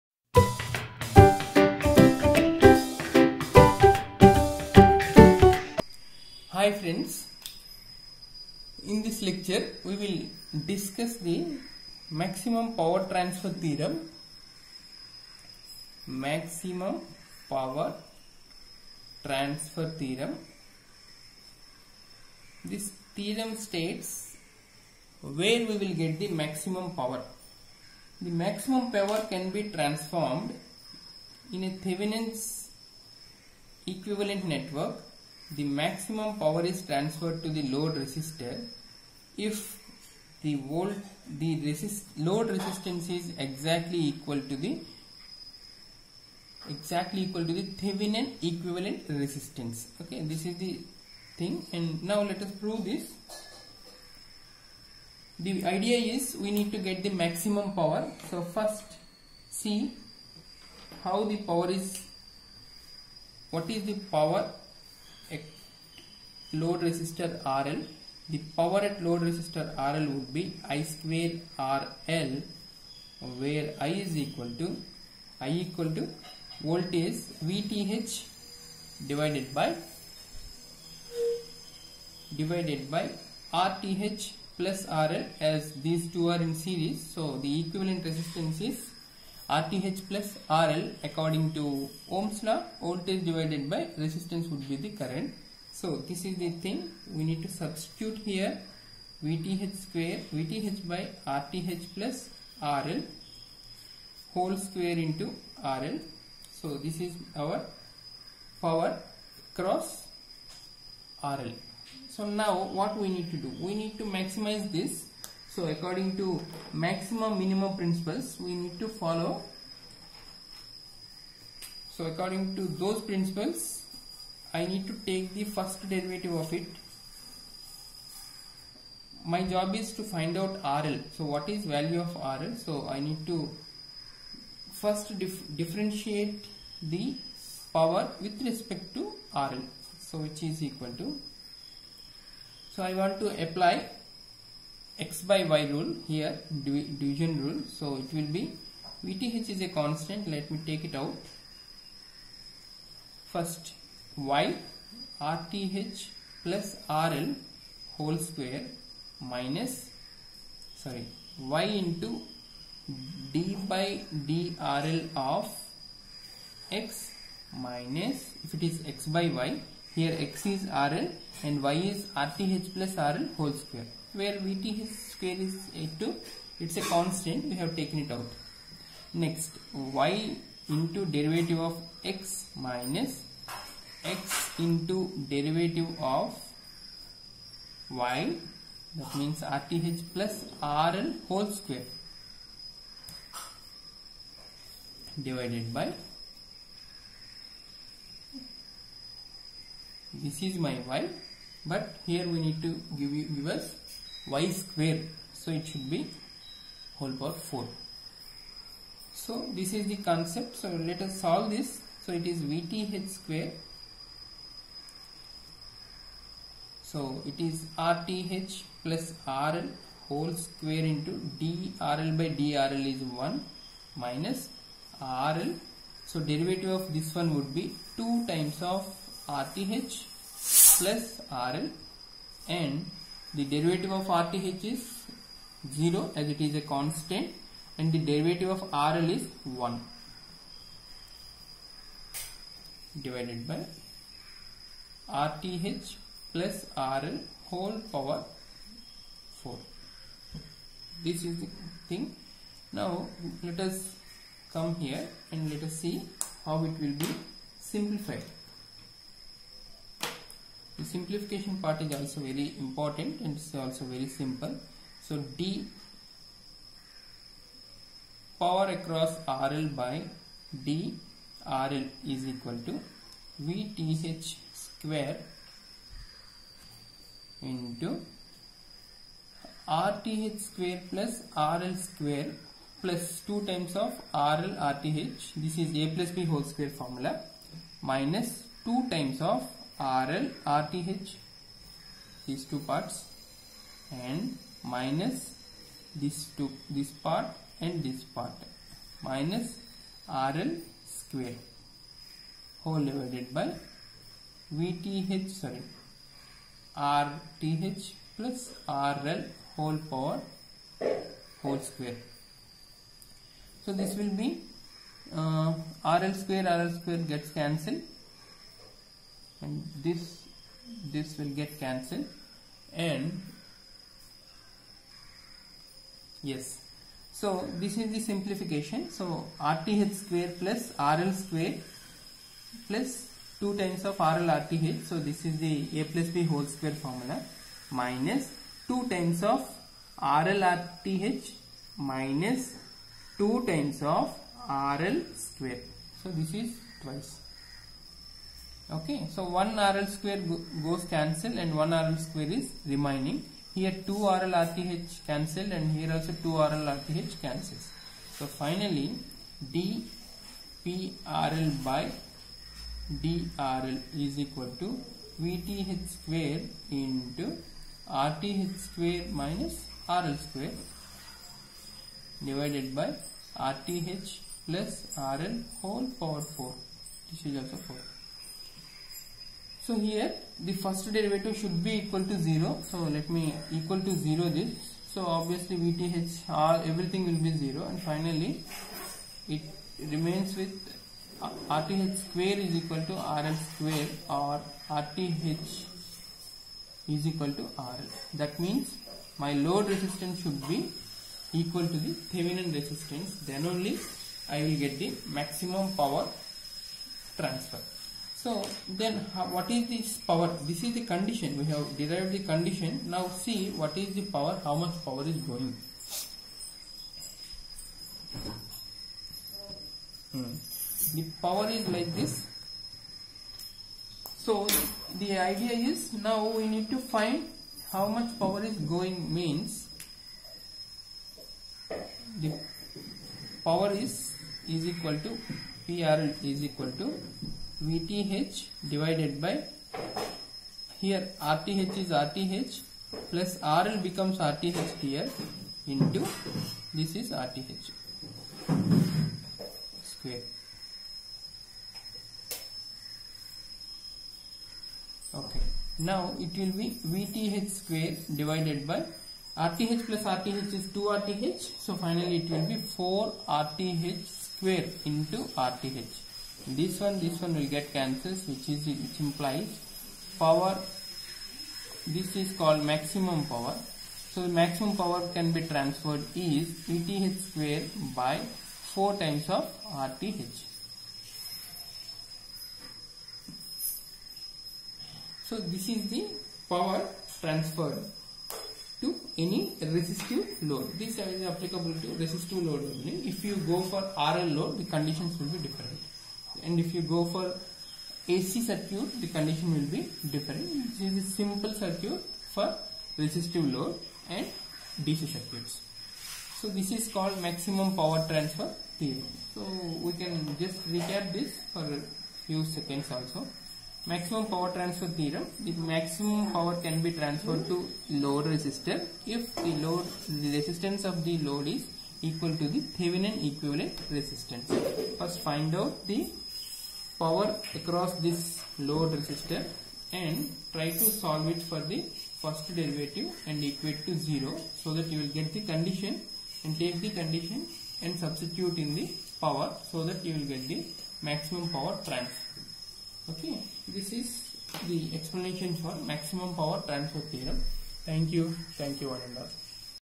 4. Maximum Power Transfer Theorem
It covers
1. Statement
2. Derivation
3. Explanation